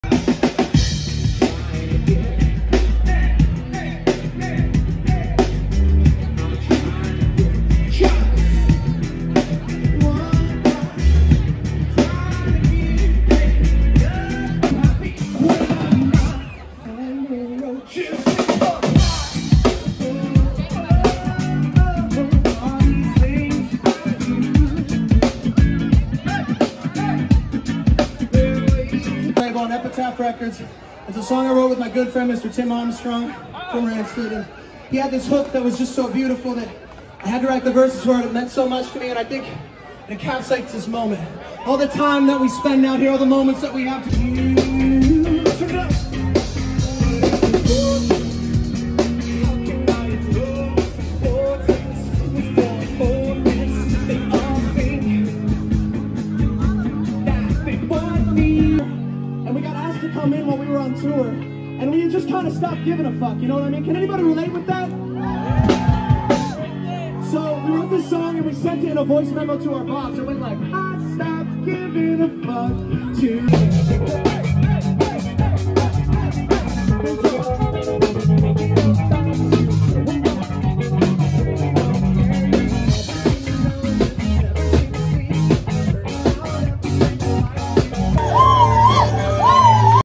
Jakobs Castle in Malibu, CA
Playing with Jakobs Castle at Kristy’s village cafe in Malibu, CA

7/28/2023